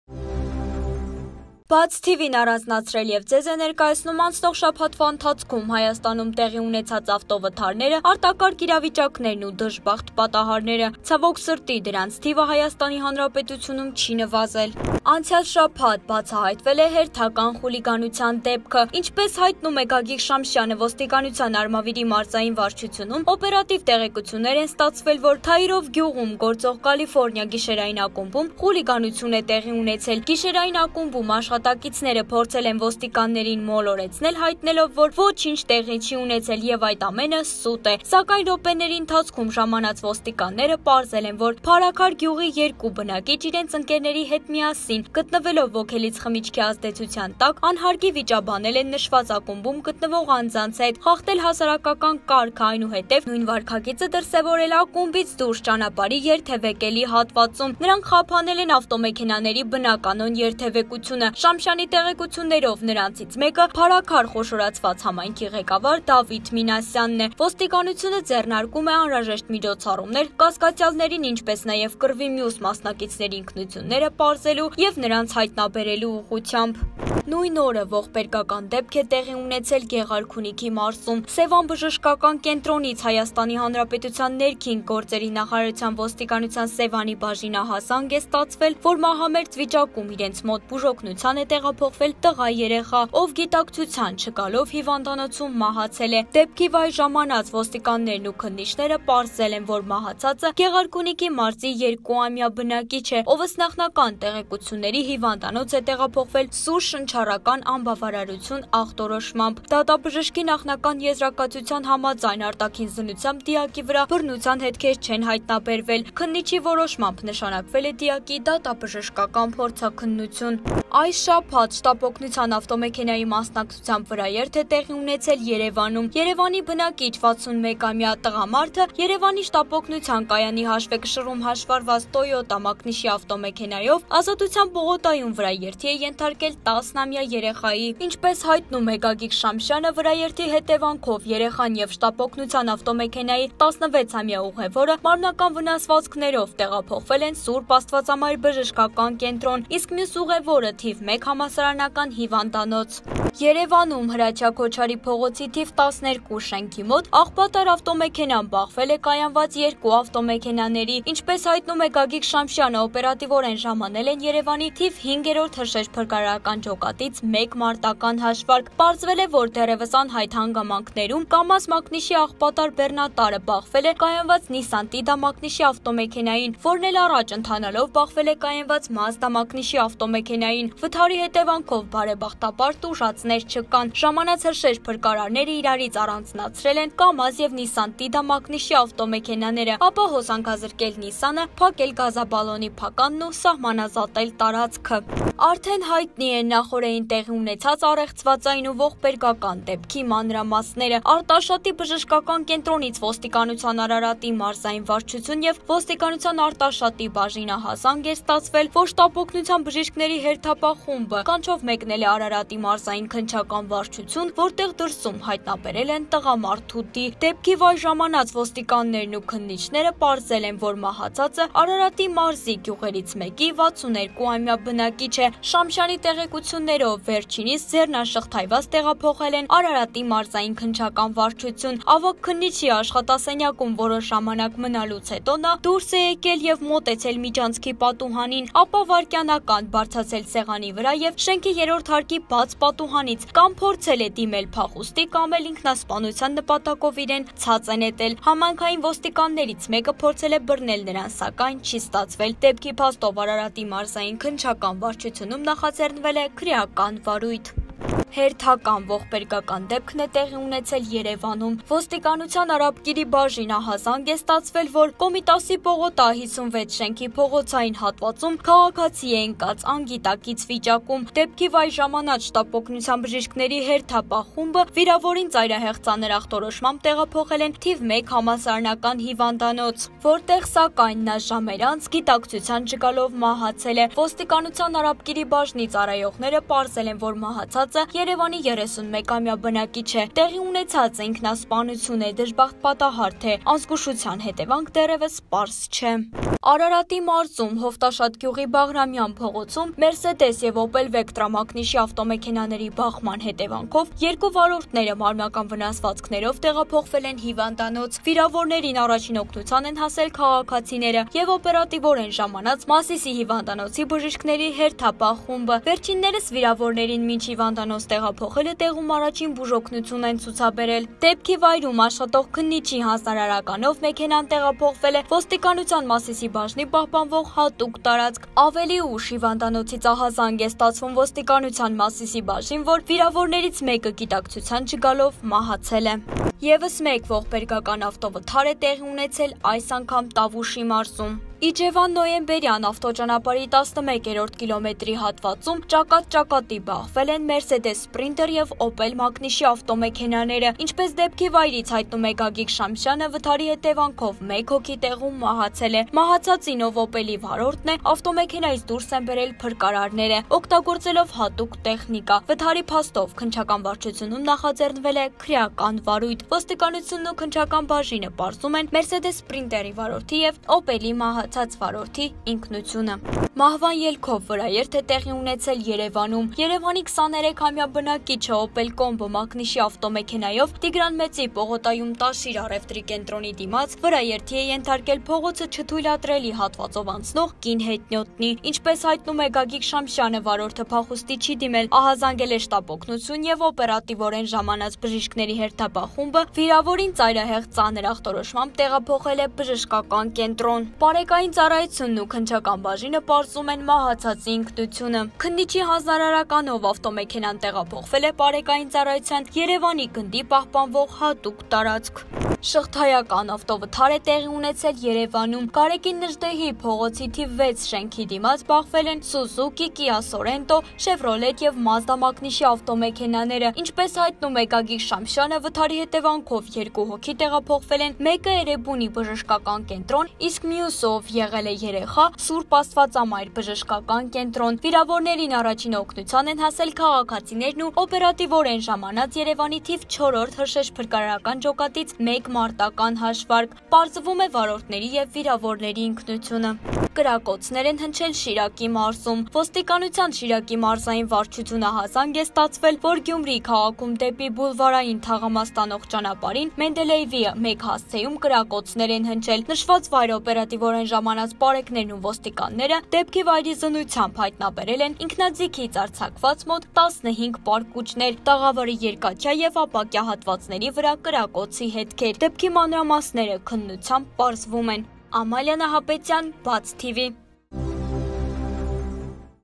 Մահացել է 2-ամյա երեխա․ Շաբաթվա քրեական քրոնիկոնը
Ողբերգական դեպք է եղել Գեղարքունիքի մարզում։ «Սևան» բժշկական կենտրոնից ՀՀ Ներքին գործերի նախարարության Ոստիկանության Սևանի բաժին ահազանգ է ստացվել, որ մահամերձ վիճակում իրենց մոտ բուժօգնության է տեղափոխվել տղա երեխա, ով գիտակցության չգալով, մահացել է։ Դեպքի վայր ժամանած ոստիկաններն ու քննիչները պարզել են, որ մահացածը Գեղարքունիքի մարզի 2-ամյա բնակիչ է, ով ըստ նախնական տեղեկությունների՝ հիվանդանոց է տեղափոխվել «սուր շնչառական անբավարարություն» ախտորոշմամբ։ Դատաբժշկի նախնական եզրակացության համաձայն՝ արտաքին զննությամբ՝ դիակի վրա բռնության հետքեր չեն հայտնաբերվել։ Քննիչի որոշմամբ՝ նշանակվել է դիակի դատաբժշկական փորձաքննություն։